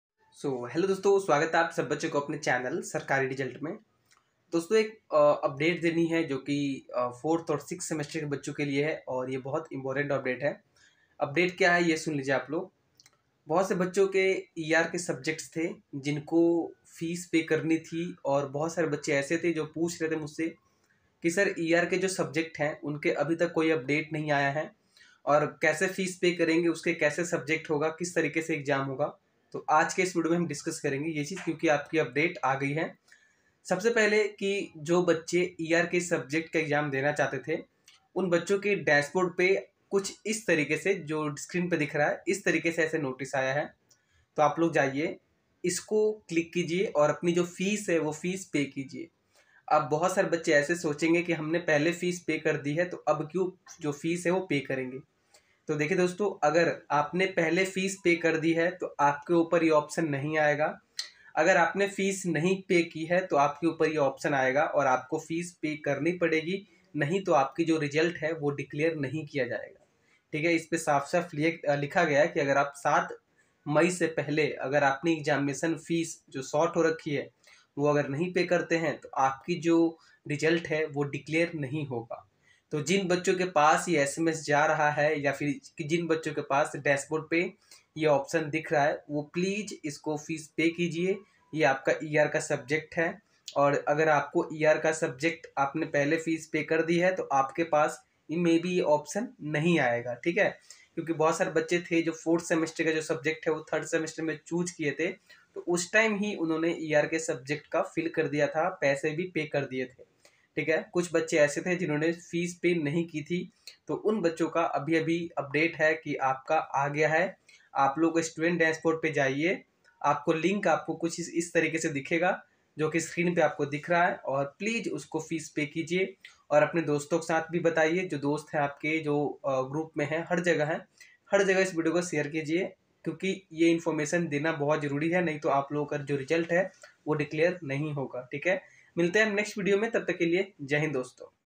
Important Video 4th & 6th Semester ER For fill & Fees Pay | Du Sol ER Subiect Form fill up
Important Video 4th & 6th Semester ER For fill & Fess Pay | Du Sol ER Subiect Form fill up

In this video we will discuss about Du sol 4th & 6th Semester Students now pay their ER Subiects for fill up and AB Subjects choice and pay.